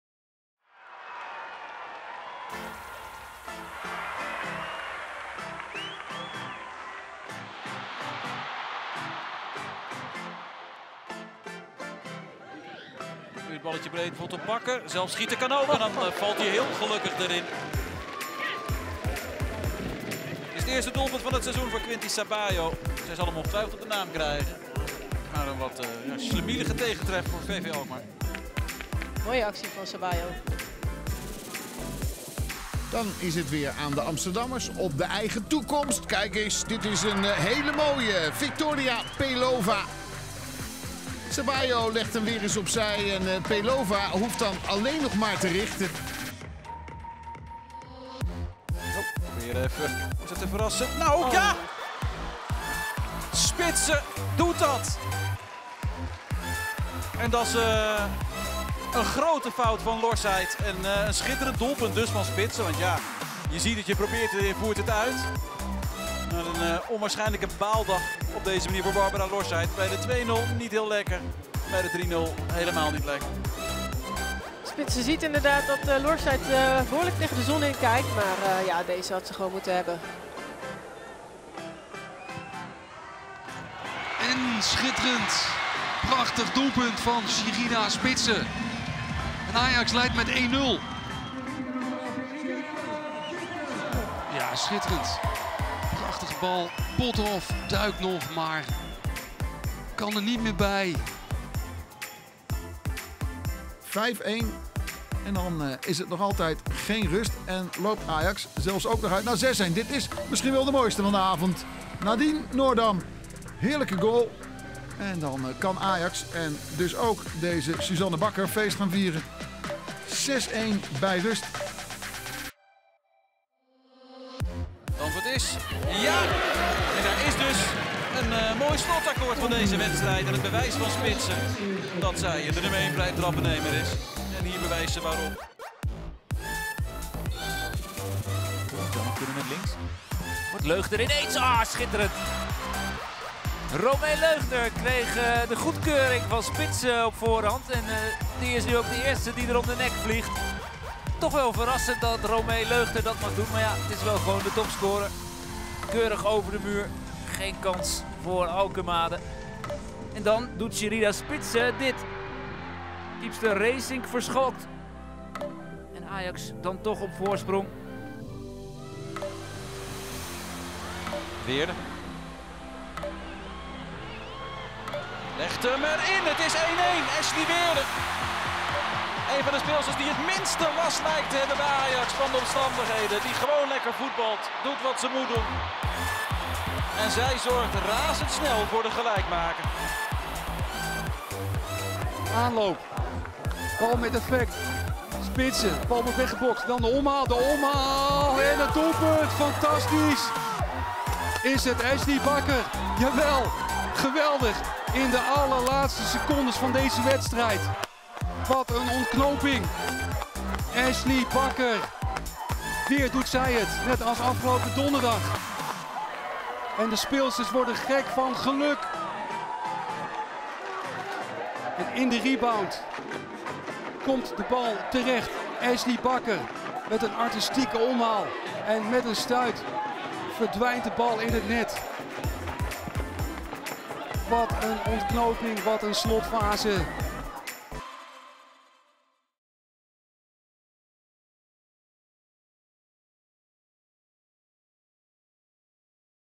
TOP 10 GOALS AJAX VROUWEN | Season 22/23 | Three stars for our captain ⭐️⭐️⭐️
De 10 mooiste doelpunten gemaakt door Ajax Vrouwen in het seizoen 2022/23.